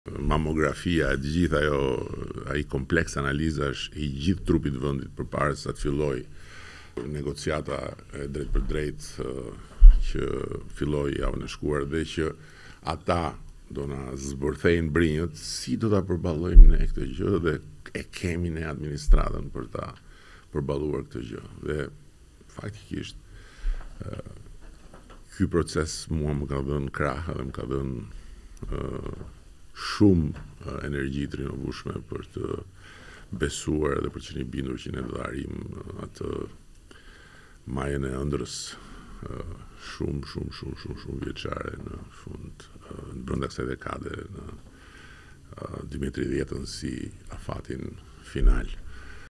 Top News/ Rama-Kisha frikë se për negociatat do na zbëthenin brinjët
Shqipëria rankohet më lart se vendet e Ballkanit Perëndimor kur vjen puna tek kuadri institucional lidhur me prokurimin publik..Ishte ky fokusi I bisedës gjatë Podkastit ‘Flasim’ të kryeministrit Rama me drejtoreshën e Agjencisë së Prokurorimit Publik, Reida Kashta si dhe kreun e Komisionit të Prokurimit Publik,Jonaid Myzyri

Me tabela e dokumente në duar Rama rrëfeu punën e madhe që u bë por edhe si dhe pse dolën me sukses në përgatitjen e hapjes së negociatave